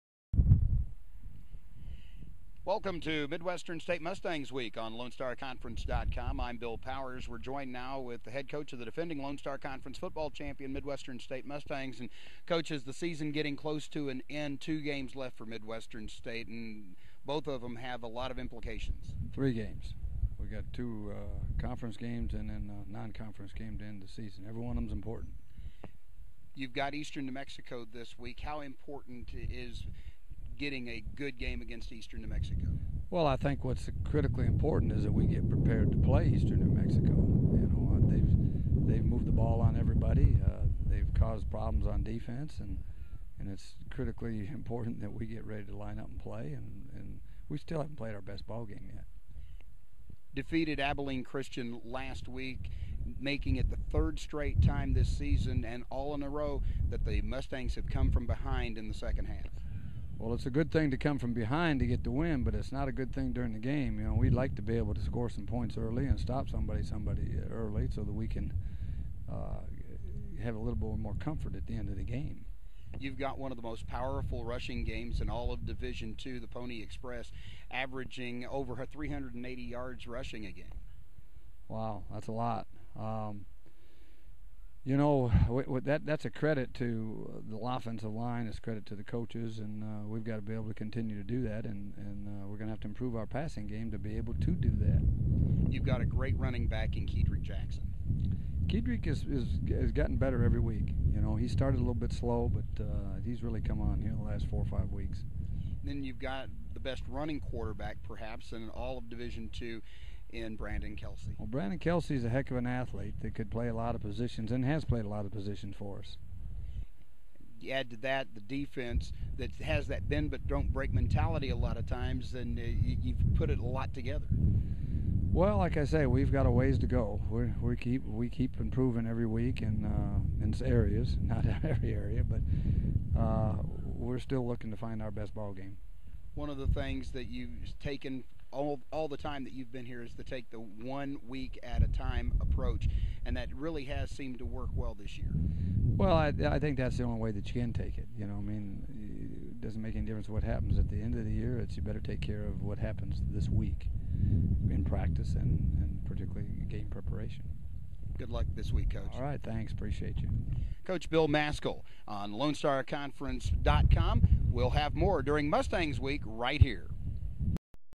MSU Mustangs Week on LoneStarConference.org
Bill Powers interviews MSU football coach Bill Maskill.